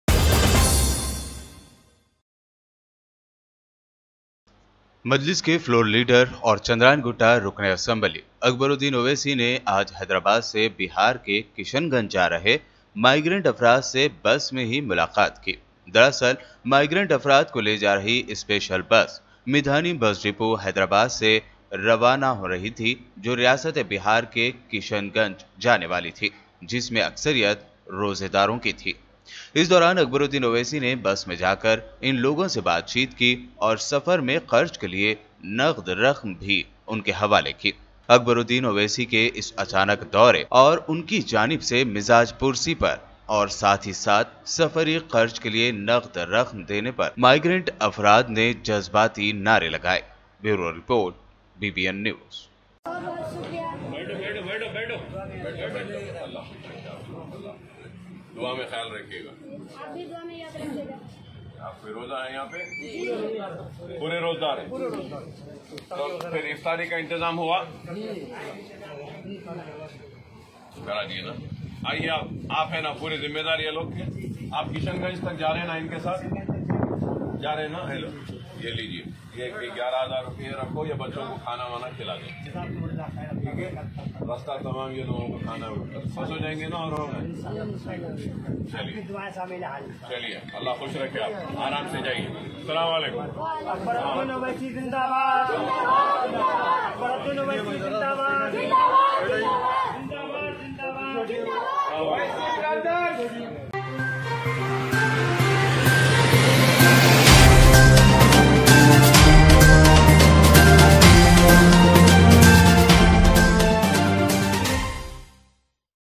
Akbaruddin Owaisi ne Bihar ke Kishanganj Jaane Wale Migrants se ki Mulaqat, Safar ke dauran Kharch
Akbaruddin Owaisi ne Bihar ke Kishanganj Jaane Wale Migrants se ki Mulaqat, Safar ke dauran Kharch ke liye diya Cash | BBN NEWS

AIMIM FLOOR LEADER AKBARUDDIN OWAISI TODAY MET WITH CHILDREN'S FROM KISHANGANJ WHO WE'RE LEAVING FOR BIHAR IN A SPECIAL BUS FROM MEDHANI BUS DEPOT HYDERABAD AND ALSO GAVE THEM RUPEES 11,000 CASH FOR THEIR JOURNEY EXPENSES